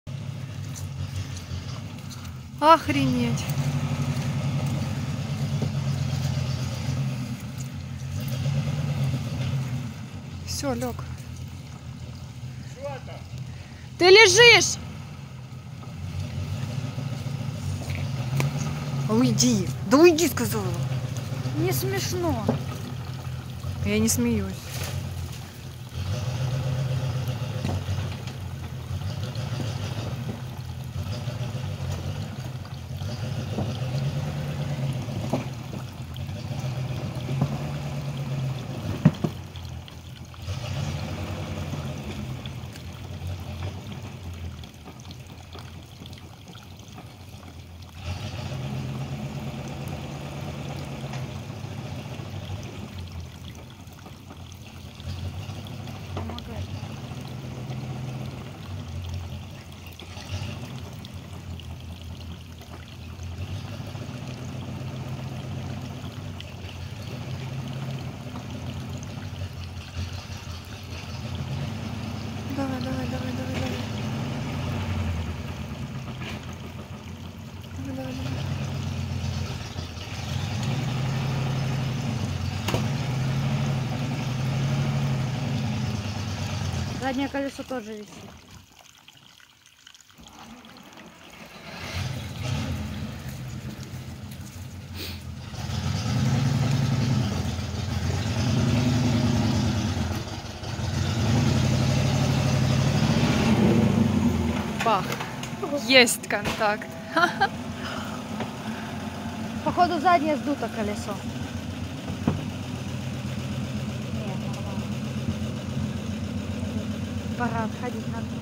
Utility Vehicle Gets Back Up After Being Stuck in a Rut || ViralHog
Occurred on January, 2019 / Krasnodar Region, Russia

"Our UAZ laid on its side but managed to get back on its wheels."